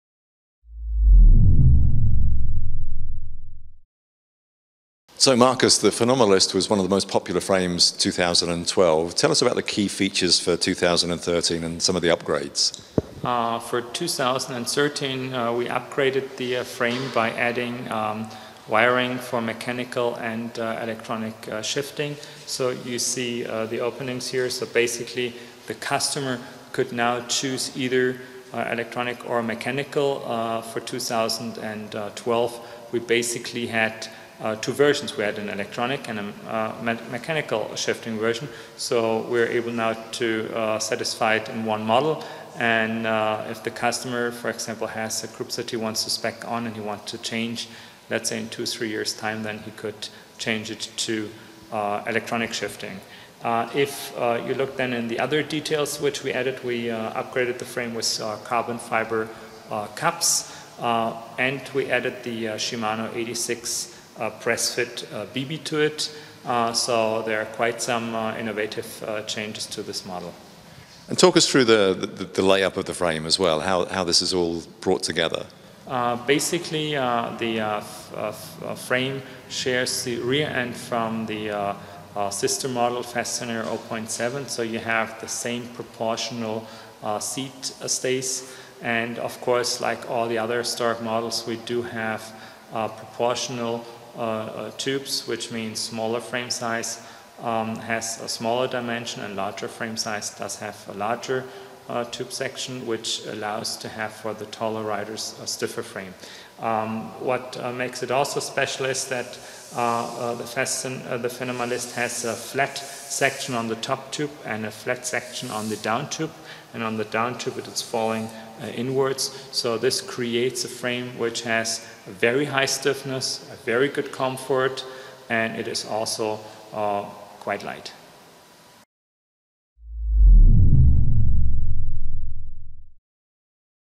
fenomalist
A classic racing machine with all the trimmings that a Storck frame set stands for: The
Fenomalist G2 in combination with the Stiletto 300 offers extremely agile steering as well as best stiffness values in the head tube and bottom bracket areas. Plus, with a frame weight from 900 grams, it provides an optimum base for a super-light speed machine. The clear-cut shapes of the Fenomalist are emphasized by modern system integration: Interior cables for electronic shifting systems plus a PressFit bottom bracket
are naturally part of the package as well as the carbon bearing shells in the head tube and bottom bracket, plus proportional tubing also for the seat stays.